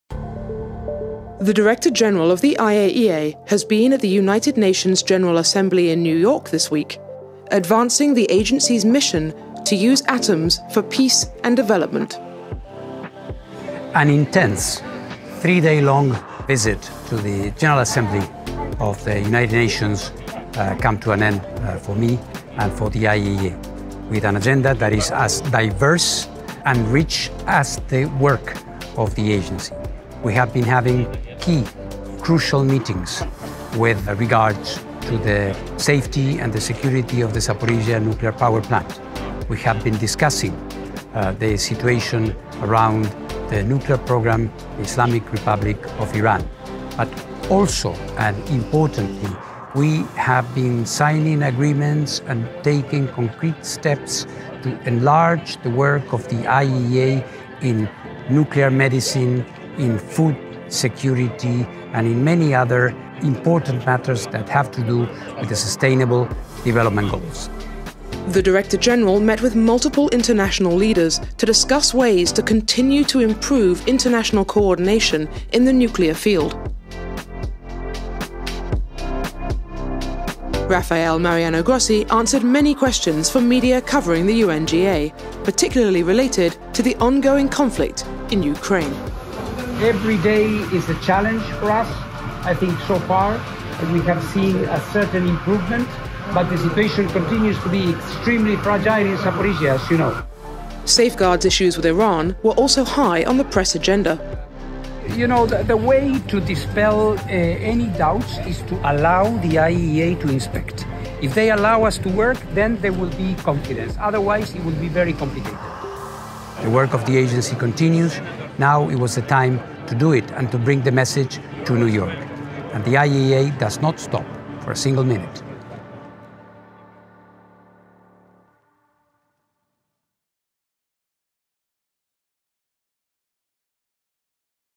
IAEA DG at the United Nations General Assembly
At UNGA, Director General @RafaelMGrossi highlighted IAEA's role for peace and development: from addressing global challenges to taking concrete steps to achieve GlobalGoals with nuclear science and technology.